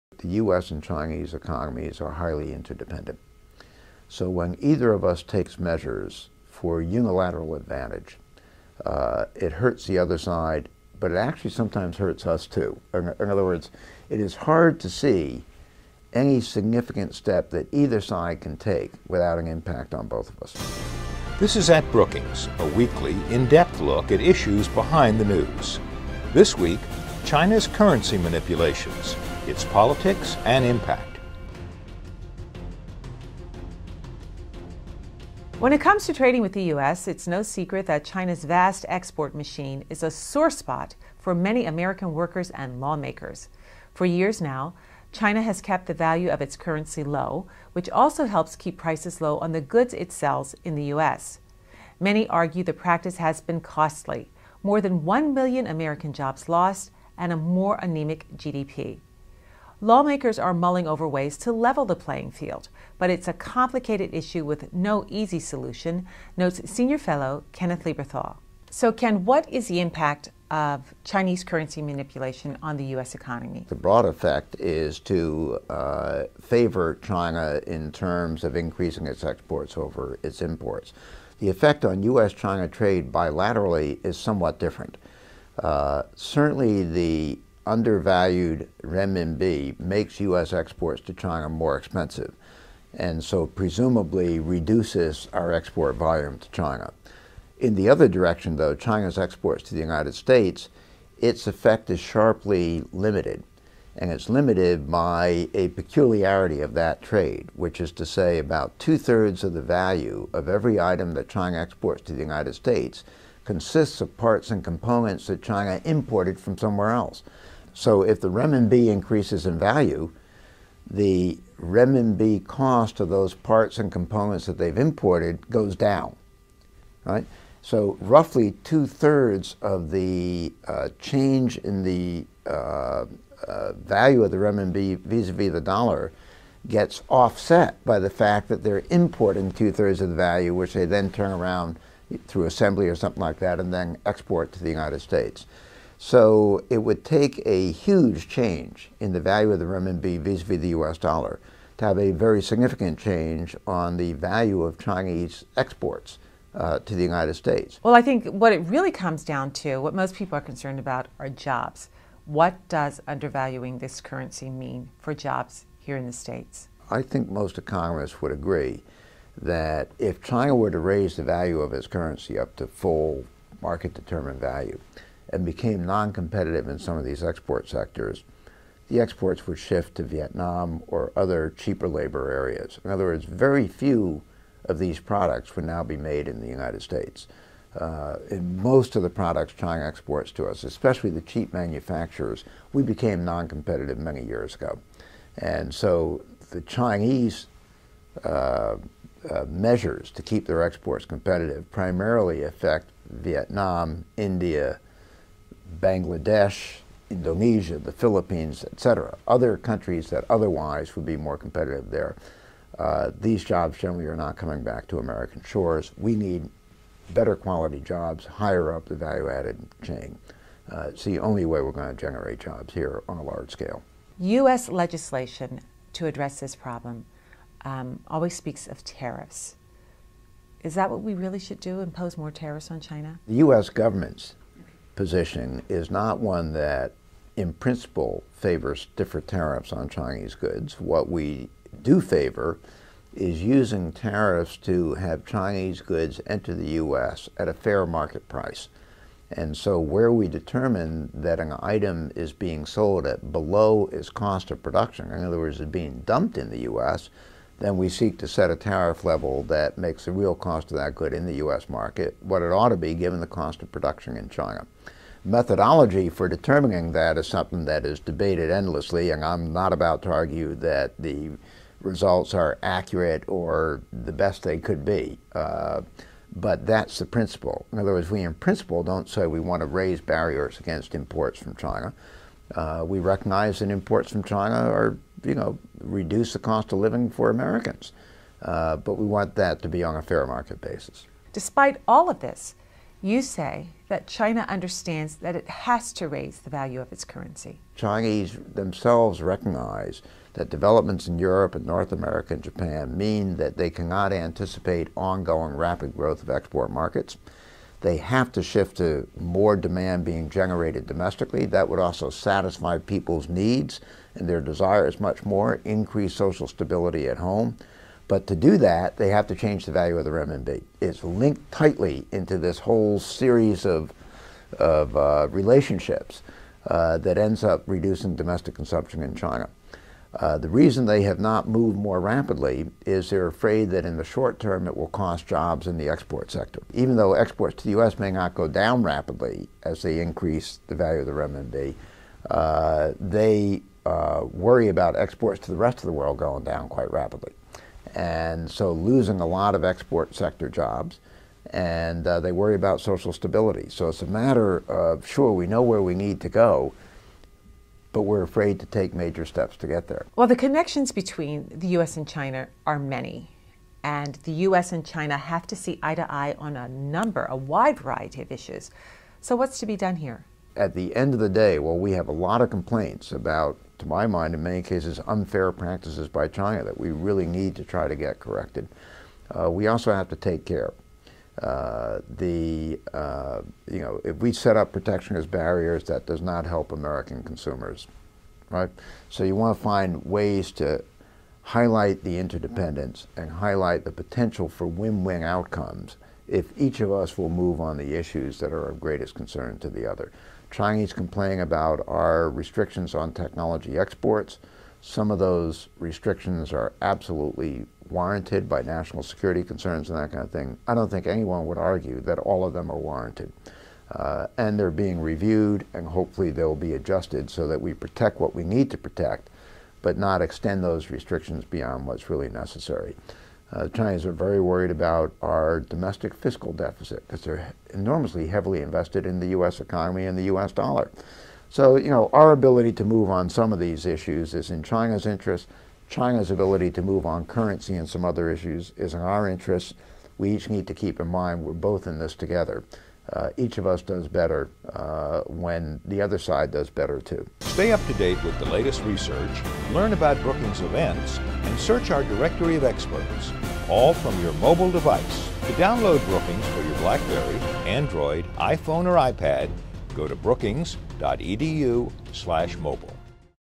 China's Currency Policy
Kenneth Lieberthal: China and the U.S. are two of the world's most important economies and it's in their mutual interest that they find a solution for the complex and critical currency problem. More